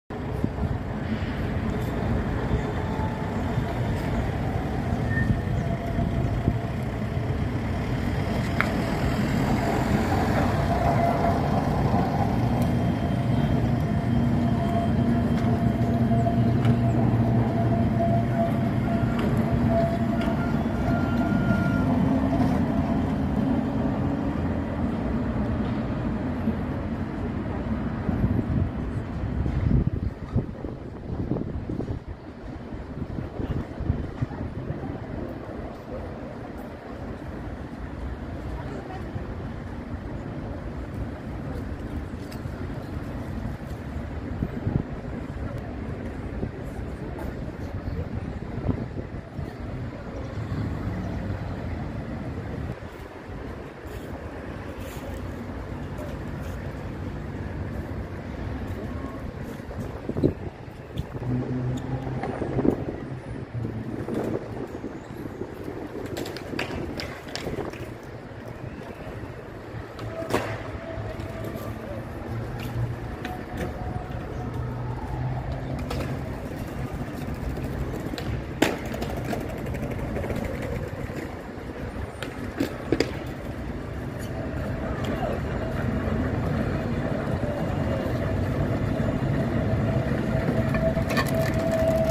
Tramways du réseau Irigo de Angers (49) et passage d'une belle limousine. @B.BRETEAU YOUTUBE.
Bonjour et bienvenue. La place Molière à Angers , où passe le tramway et une belle limousine , pour un mariage ,peut-être. Le tramway remonte la rue de la Roë, qui est à voie unique pour le tramway ,entre la Place du Ralliement et la Place Molière. La rue de la Roë est devenue semi-piétonne depuis 2010.
Merci pour toutes vos vues et à bientôt.
@B.BRETEAU YOUTUBE.